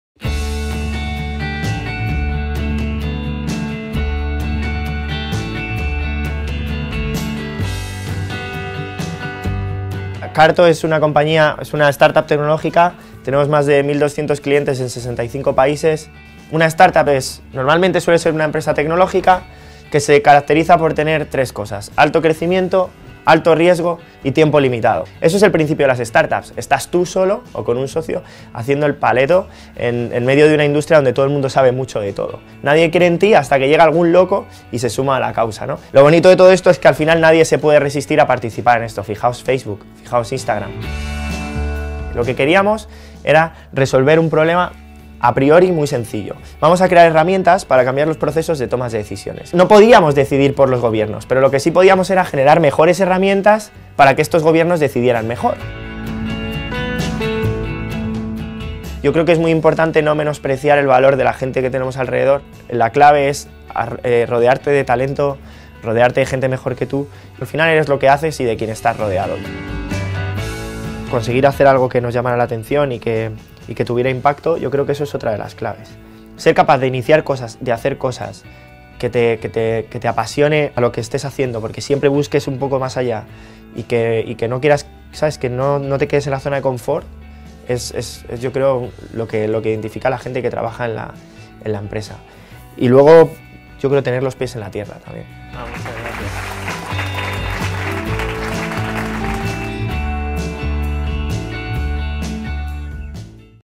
Encuentro con Sergio Álvarez Leiva
Sergio Álvarez Leiva, fundador y jefe de Producto de Carto, empresa española que plasma en mapas interactivos grandes volúmenes de datos, ha visitado la UFV para mantener un encuentro con los alumnos.